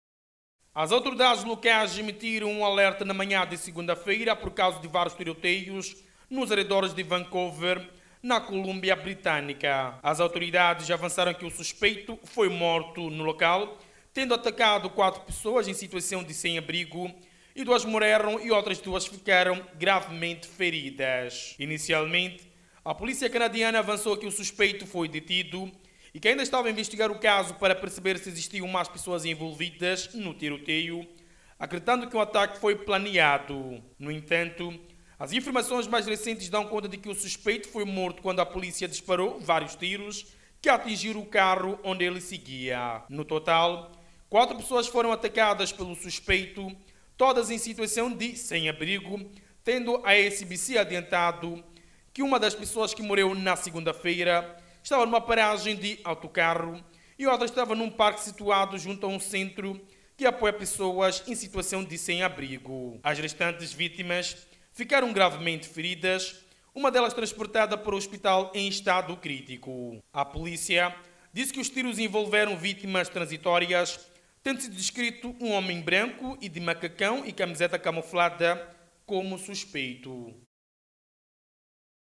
TIROTEIO CANADÁ: ATIRADOR MATA TRÊS PESSOAS SEM-ABRIGO E FERE OUTRAS NO CANADÁ
Em um alerta enviado aos telefones dos moradores de BC, a polícia disse que os tiros envolveram “vítimas transitórias”, tendo sido descrito um homem branco de macacão e camiseta camuflada como suspeito.As autoridades locais emitiram um alerta na manhã de segunda-feira por causa de vários tiroteios nos arredores de Vancouver, na Colúmbia Britânica.

As autoridades avançaram que o suspeito foi morto no local. O homem atacou quatro pessoas em situação de sem-abrigo e três morreram e outras duas ficaram gravemente feridas.